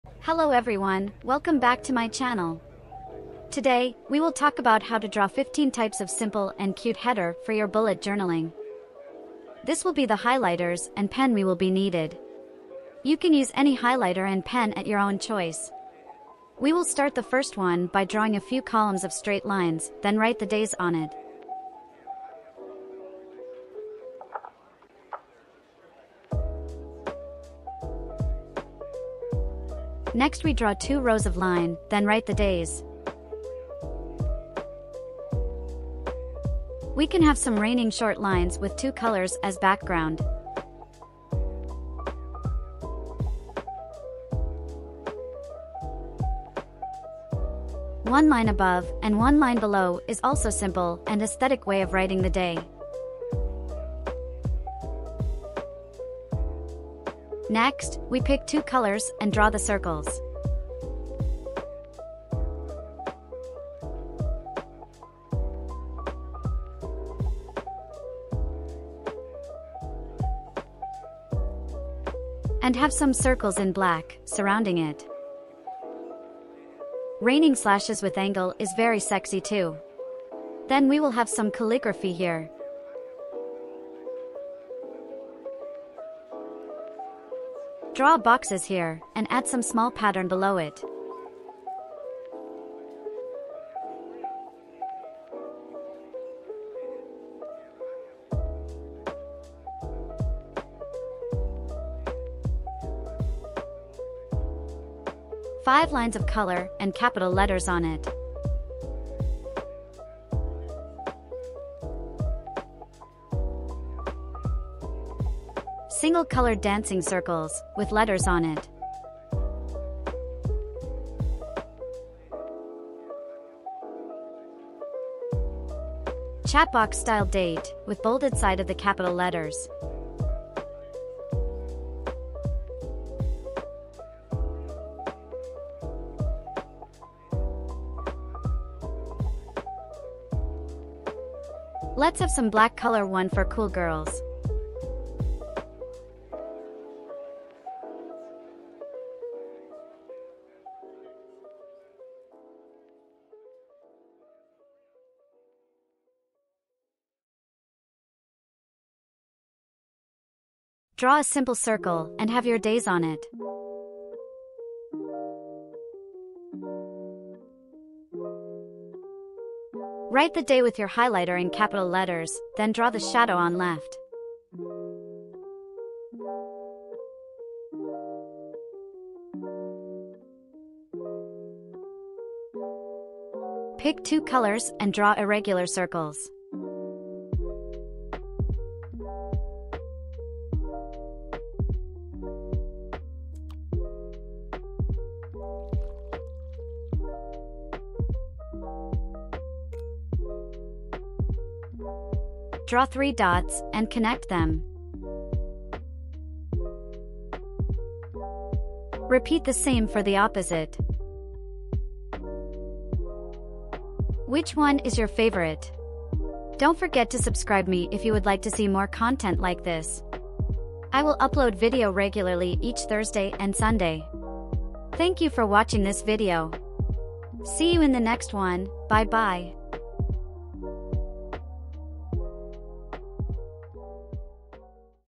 Easy Header & Title for Bullet Journaling | with Highlighters
In this video... sharing a cute and easy way to create HEADERS for your bullet journal with highlighters and pen!😀
hi friends~ This is Ydione_Journal(⊃｡•́‿•̀｡)⊃  I share videos on bullet journal, self-learning and life experiments.

MORE videos in my channel page
小红书: 奶油手账（日更中）

Timestamps:
00:00 - Intro
00:10 - Stationaries used
00:17 - First Title
01:16 - Sixth Title
02:21 - Eleventh Title
03:40 - Overview
03:45 - Thank you